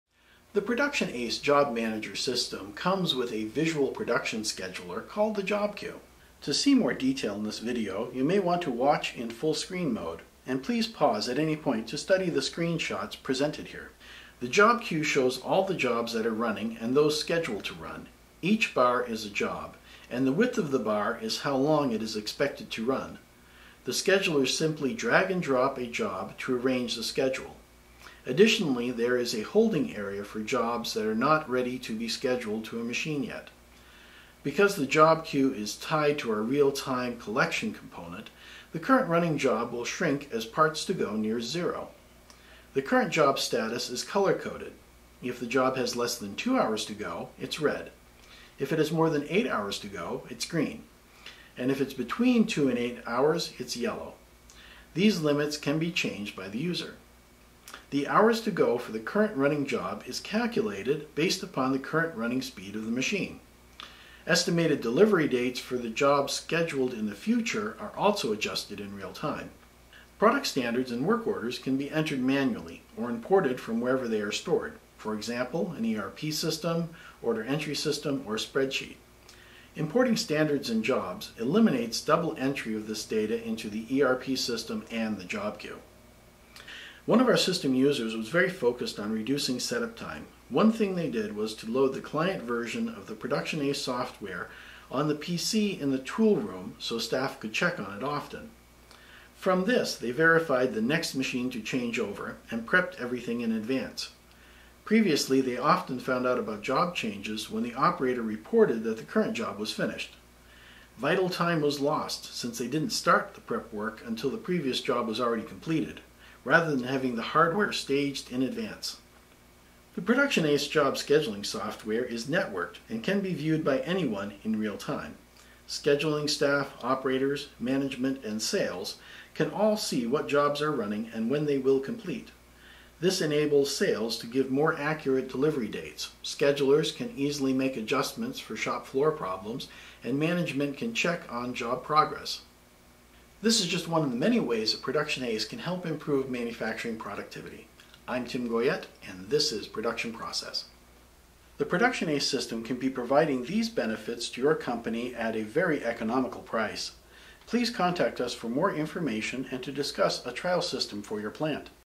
Graphical Production Scheduling and Production Planning Software
You can simplify and improve production scheduling accuracy and view actual end dates based upon the shift schedule and real-time machine performance.